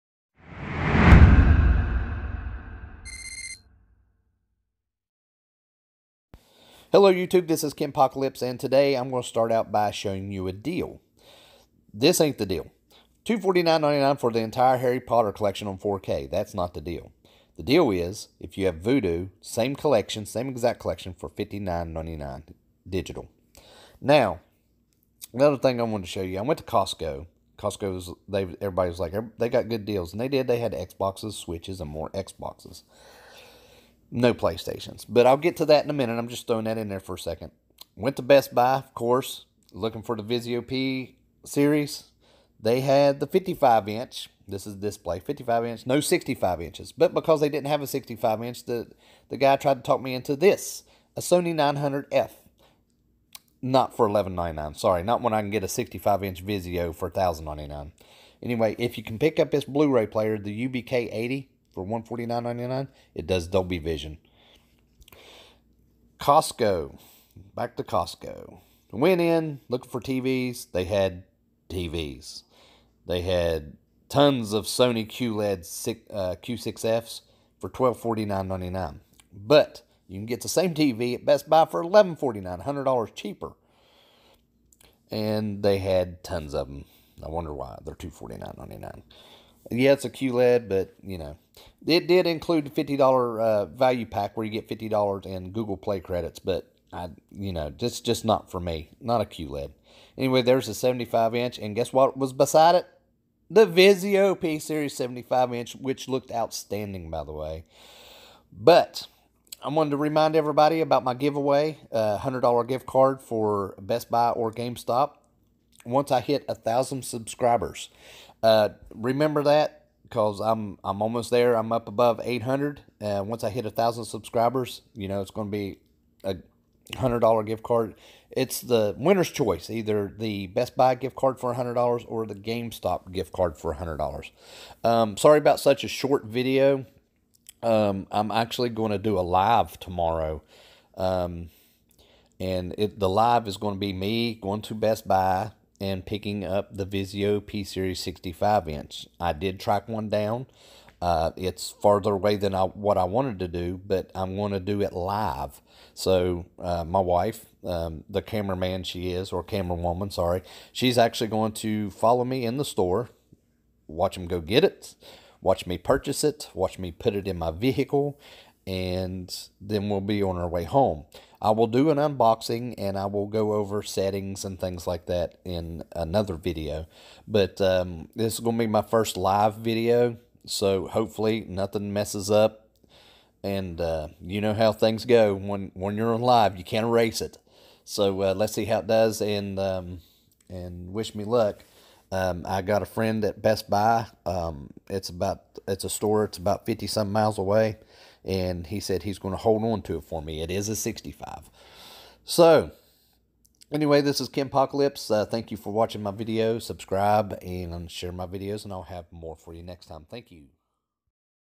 FOUND The Elusive Vizio P , and Costo Loves Samsung
Just throwing in a couple deals I found the past couple days and I found a video P series finally and went to Costco in this seems that they're in love with Samsung because that's pretty much all they carry